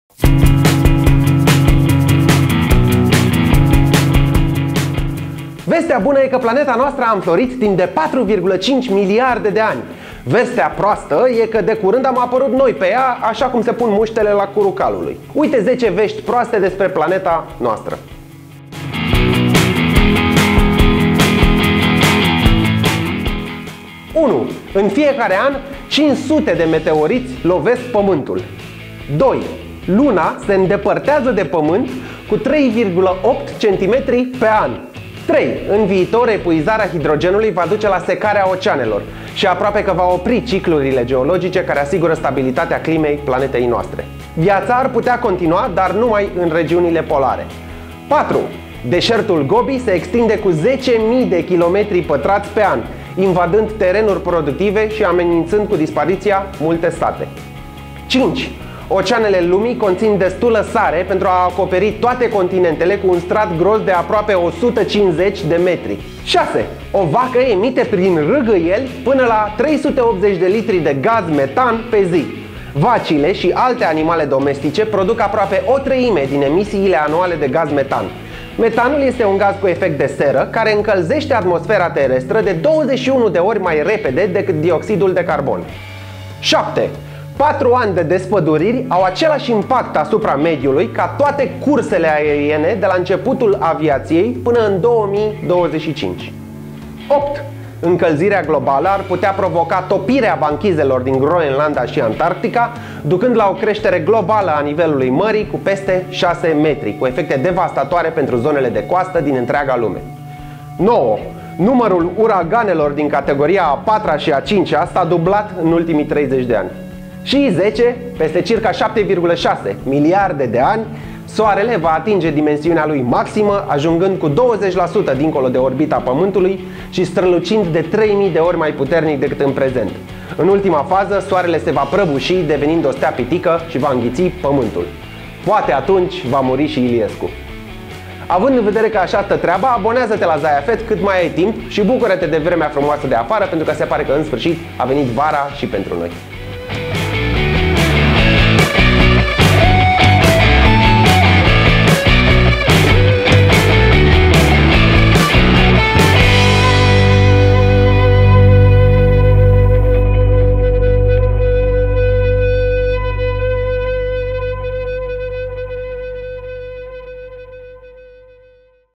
10 vești proaste despre planeta noastră
1. În fiecare an, 500 de meteoriți lovesc Pământul.
2. Luna se îndepărtează de Pământ cu 3,8 cm pe an.
3. În viitor, epuizarea hidrogenului va duce la secarea oceanelor și aproape că va opri ciclurile geologice care asigură stabilitatea climei planetei noastre. Viața ar putea continua, dar numai în regiunile polare.
4. Deșertul Gobi se extinde cu 10.000 km2 pe an, invadând terenuri productive și amenințând cu dispariția multe sate.
5. Oceanele lumii conțin destulă sare pentru a acoperi toate continentele cu un strat gros de aproape 150m
6. O vacă emite prin râgâieli până la 380 de litri de gaz metan pe zi. Vacile și alte animale domestice produc aproape o treime din emisiile anuale de gaz metan. Metanul este un gaz cu efect de seră, care încălzește atmosfera terestră de 21 de ori mai repede decât dioxidul de carbon.
7. Patru ani de despăduriri au același impact asupra mediului ca toate cursele aeriene de la începutul aviației până în 2025.
8. Încălzirea globală ar putea provoca topirea banchizelor din Groenlanda și Antarctica, ducând la o creștere globală a nivelului mării cu peste 6 m, cu efecte devastatoare pentru zonele de coastă din întreaga lume.
9. Numărul uraganelor din categoria a IVa și a Va s-a dublat în ultimii 30 de ani.
10. Peste circa 7,6 miliarde de ani, soarele va atinge dimensiunea lui maximă, ajungând cu 20% dincolo de orbita Pământului și strălucind de 3000 de ori mai puternic decât în prezent. În ultima fază, soarele se va prăbuși, devenind o stea pitică, și va înghiți Pământul. Poate atunci va muri și Iliescu.